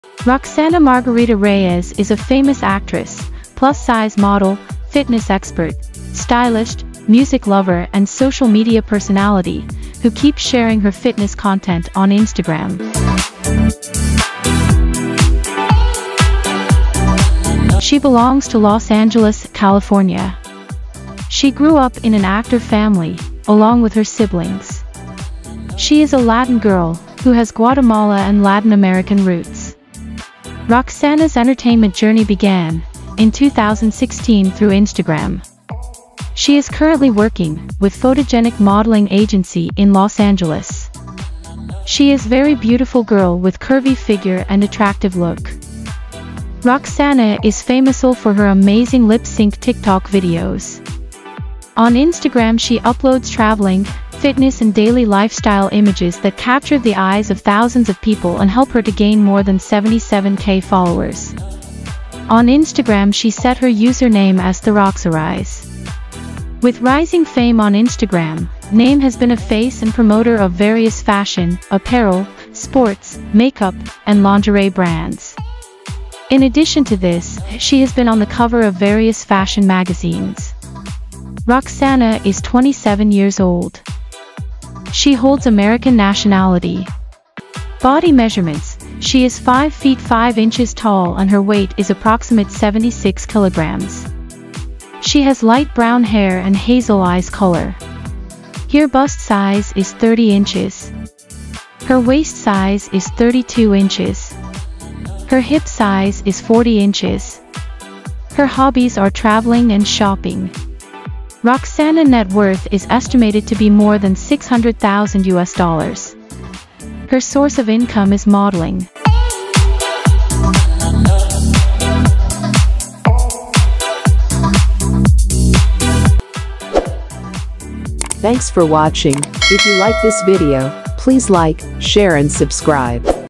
Roxana Margarita Biography | Lifestyle | Age | Body Measurements | Boyfriend | Career | PLussize
Roxana Margarita is an American actress, plus-size model, and social media personality. She is well known for her appearances in various magazines and movies.

Today in this video you will get to know all about Roxana Margarita's Biography, Lifestyle, Age, Height, Net Worth, career, Family, Early Life, and many more things.

0:00 - Roxana Margarita Intro
0:17 - Roxana Margarita Biography
0:19 - Roxana Margarita Birth Place
1:22 - Roxana Margarita Age
1:27 - Roxana Margarita Body Measurements
1:52 - Roxana Margarita's Net Worth